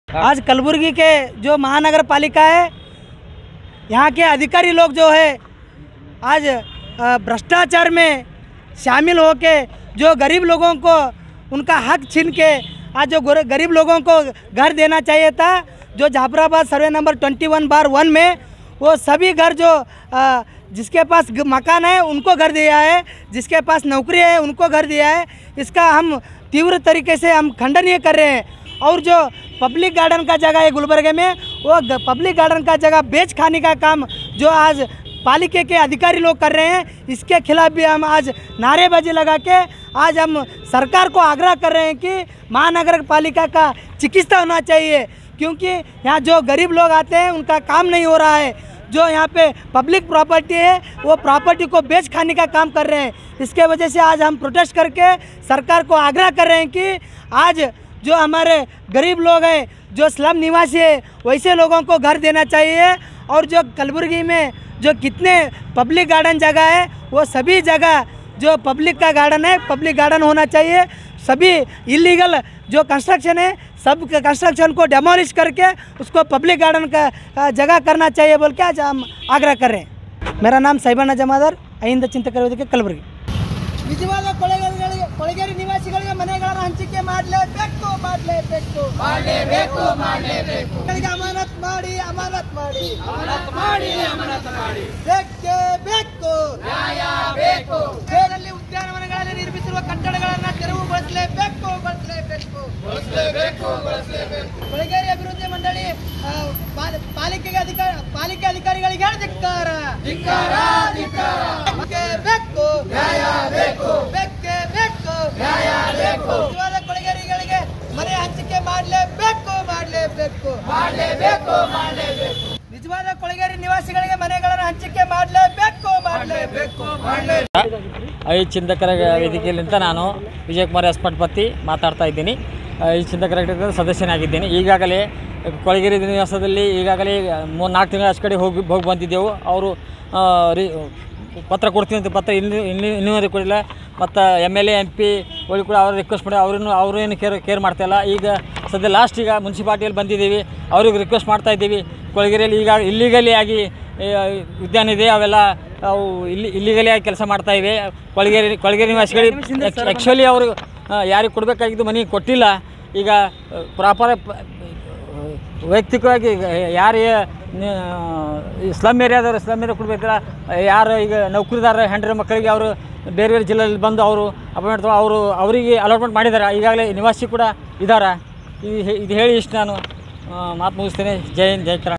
Gulbarga Ahinda Chintakaya Vedike Protest At Kalaburagi Mahanagar Palike Office & Demand to Adm...
Gulbarga Ahinda Chintakaya Vedike Members Protest At Kalaburagi Mahanagar Palike Office & Demand to Administration Take Action Against City Corporation Office Staff